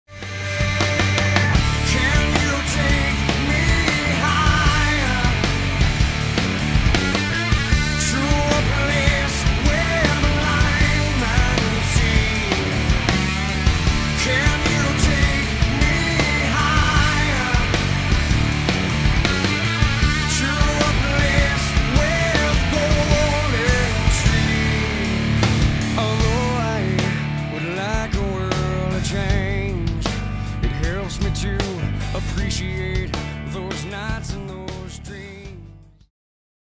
Scott Stapp - The Voice of Creed: Nov. 22nd @ The Paramount
Don't Miss Scott Stapp - The Voice of Creed playing live in concert at The Paramount in Huntington on Nov. 22nd with Special Guests - Adelitas Way, Citizen Zero & MANAFEST